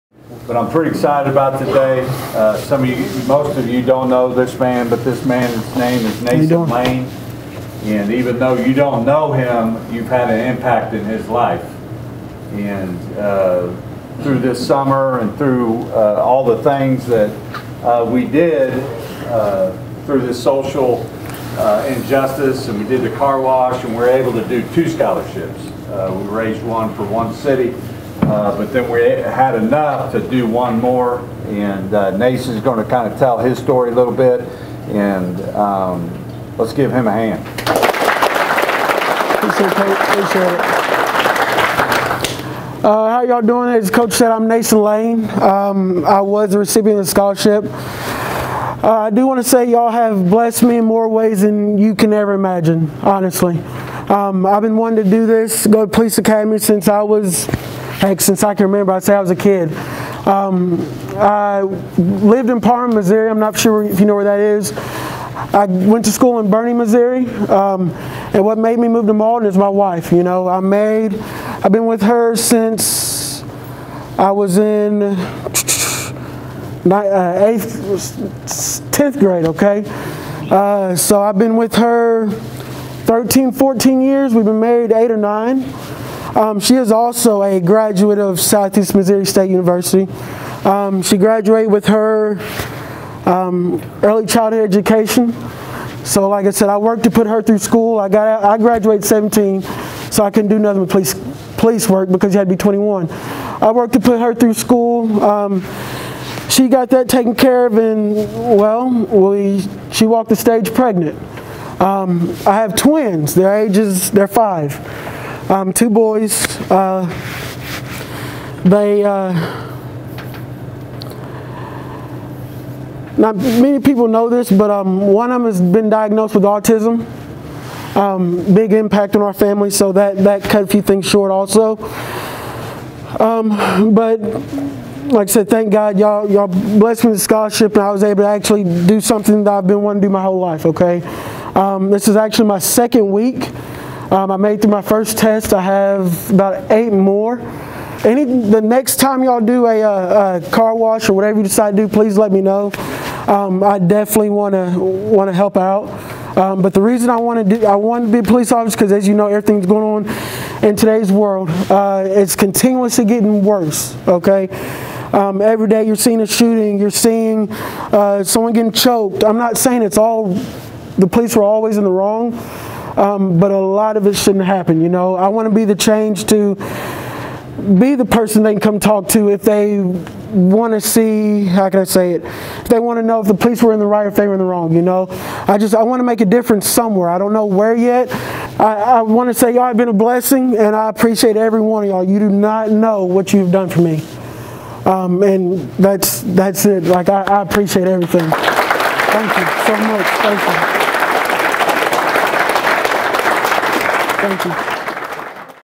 SEMO Football | Law Enforcement Scholarship Recipient Nason Lane visits the Redhawks
Nason Lane, the recipient of the inaugural Southeast Missouri Football Law Enforcement Academy Scholarship last month, paid a visit to SEMO Football to tell his story and show his gratitude.